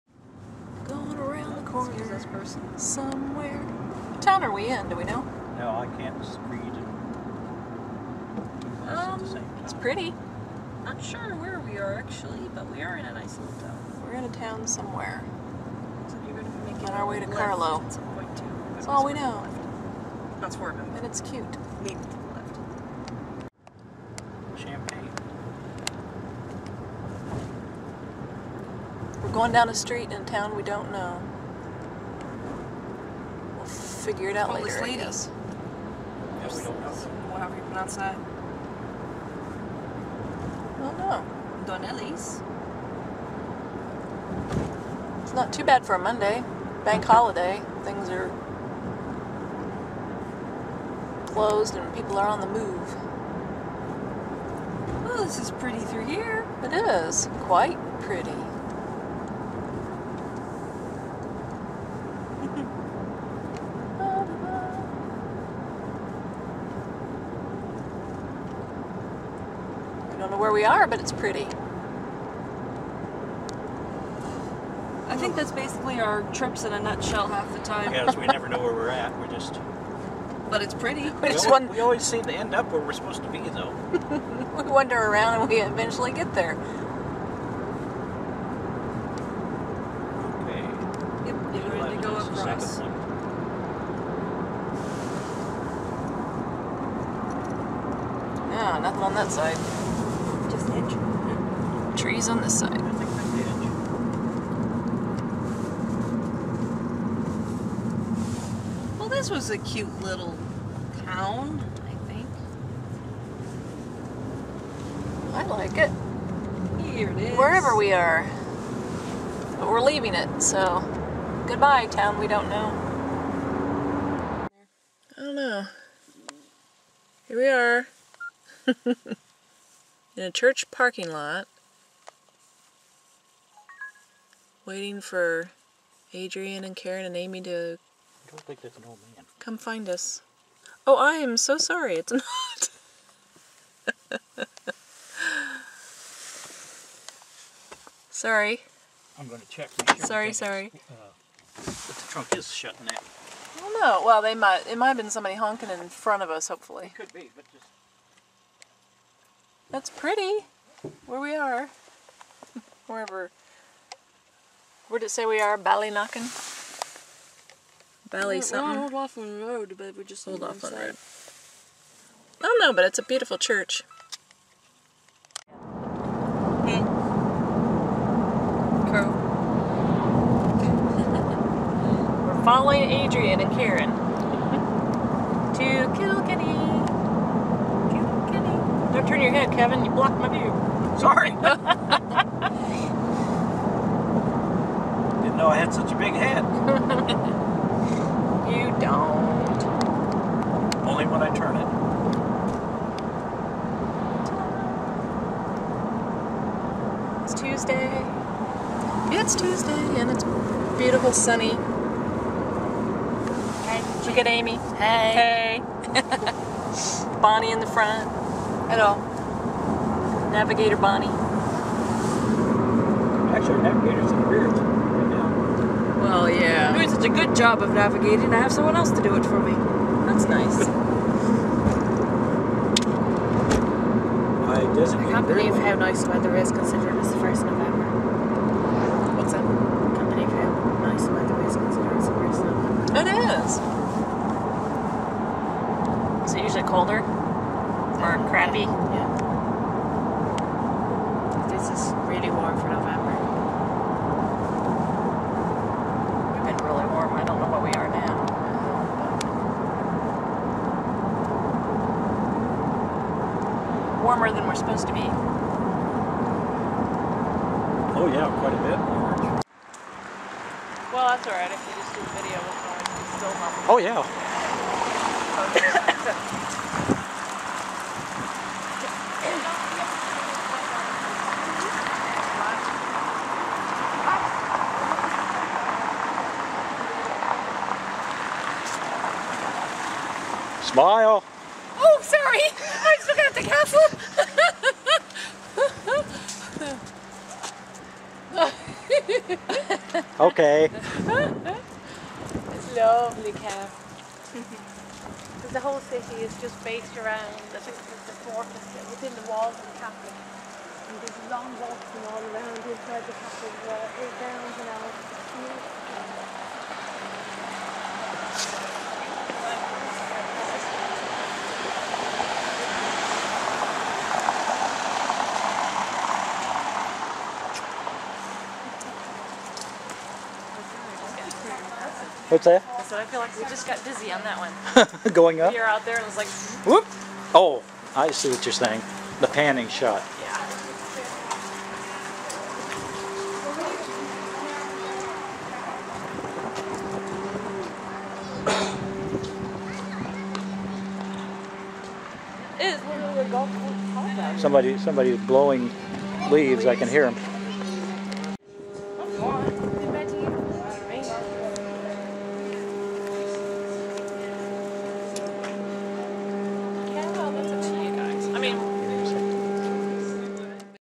Ierland travel2
Traveling to the Rowlett's and then to Kilkenny